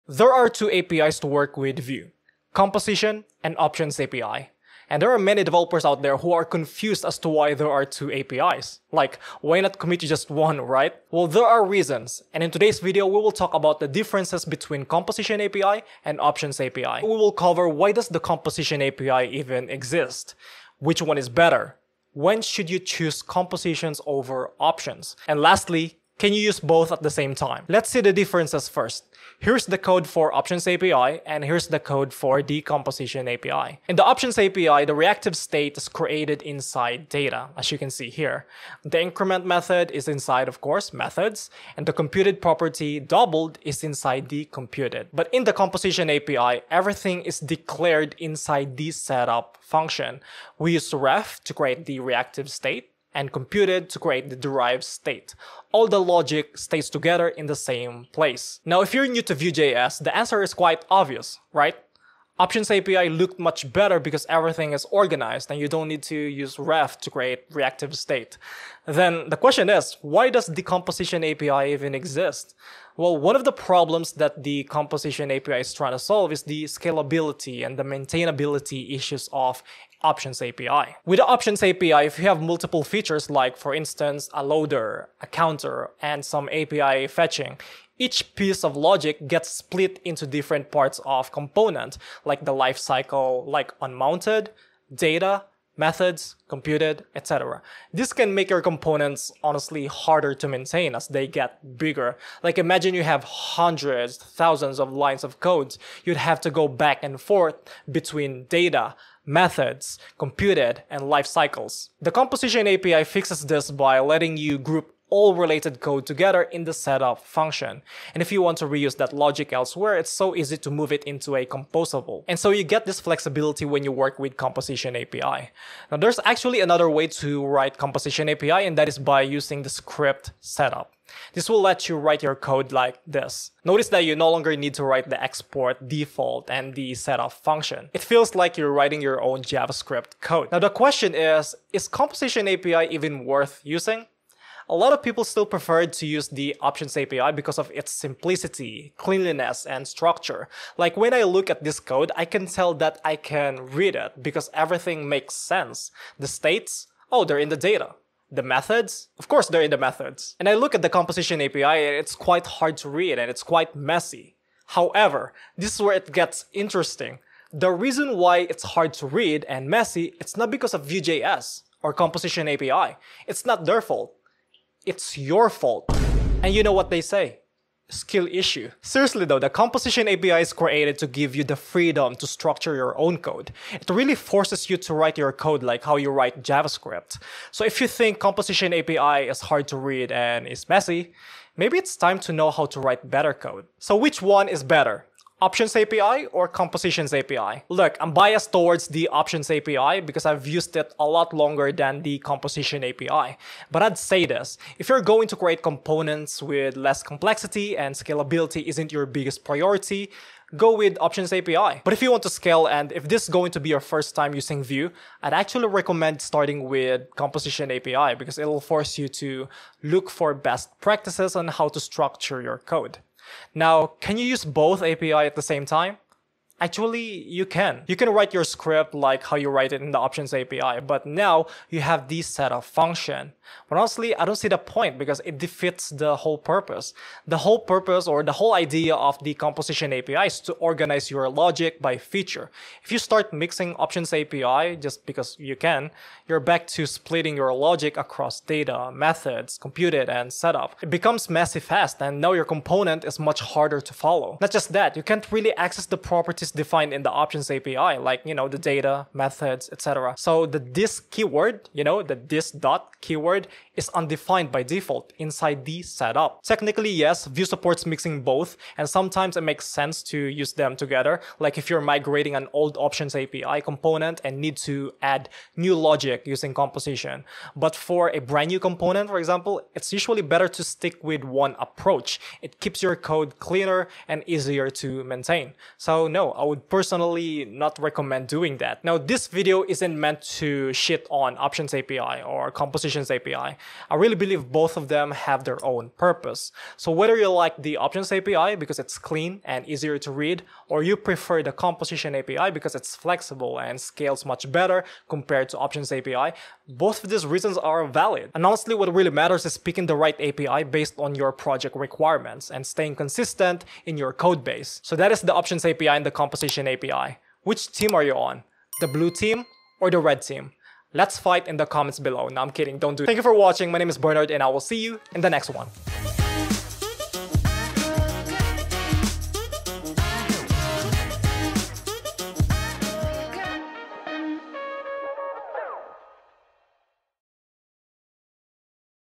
You’re Writing Vue Wrong — Use This Instead (Options vs Composition API)
Vue.js has two APIs: Options API and Composition API.
But why? Why didn’t Vue just pick one? Why composition is invented? And which one should you use?

In this video, I’ll break down the real differences between Composition vs Options API:

✅ Why both exist
✅ Which one is better for beginners vs big projects
✅ Why mixing them actually makes your code messy